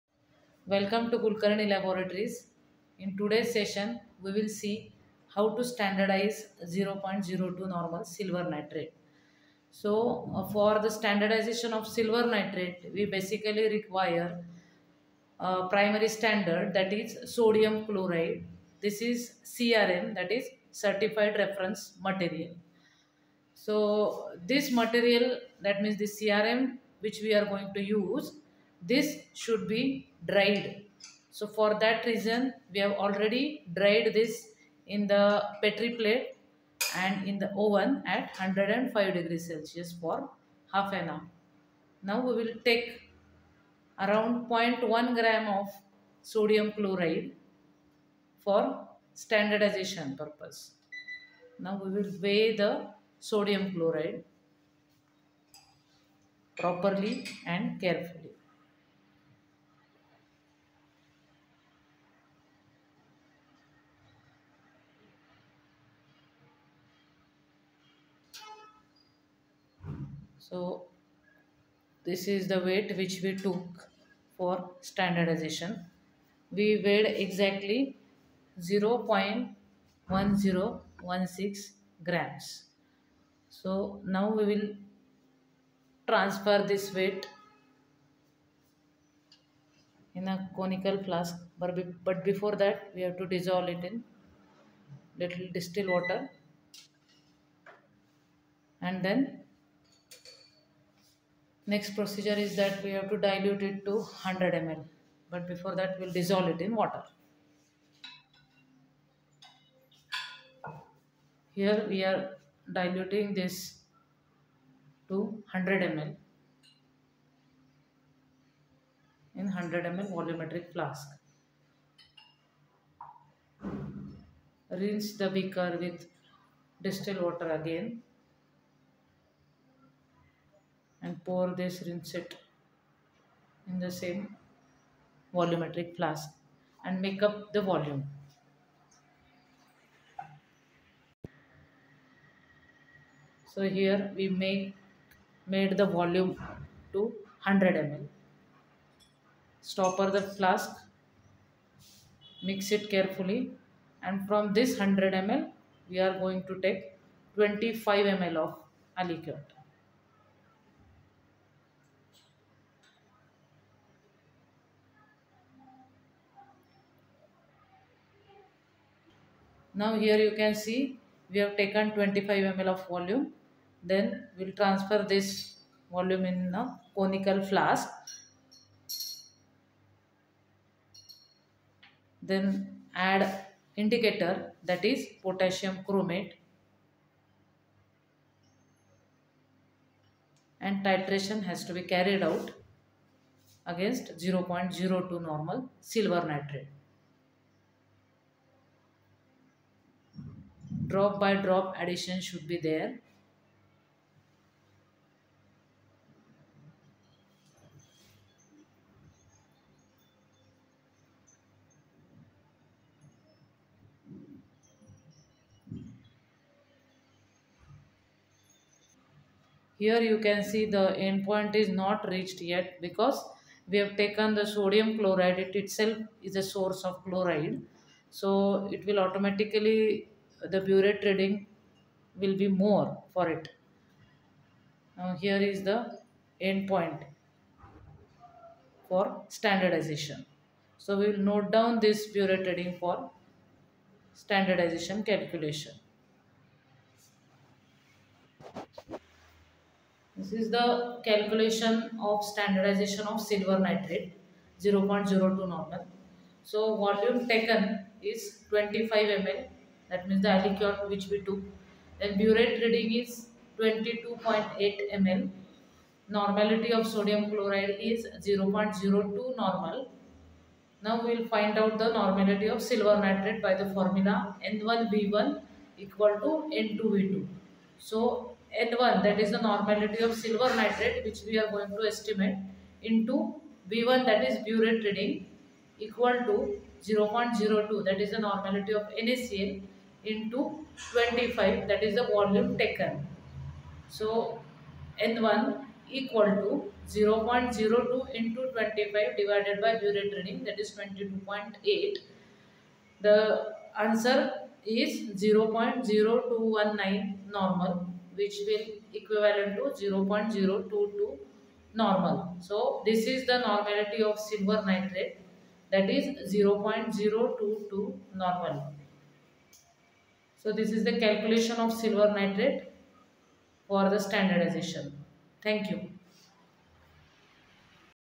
Standardization of Silver Nitrate (AgNO3)
It is the basic and easy method for the standardization of Silver Nitrate by using Sodium Chloride (NaCl).